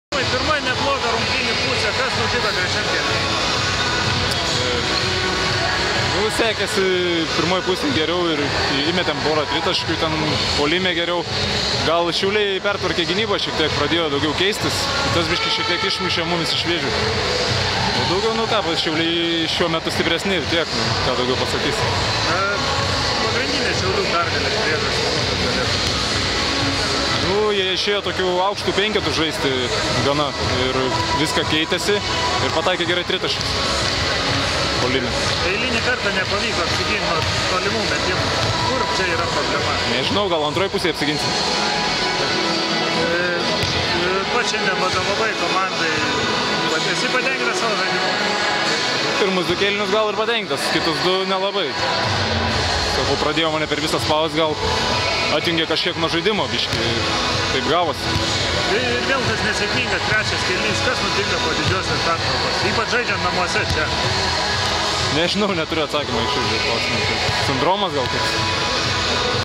T.Gaidamavičius: "Šiauliai" šiuo metu stipresni ir tiek
Tomo Gaidamavičiaus interviu po pralaimėjimo "Šiaulių" klubui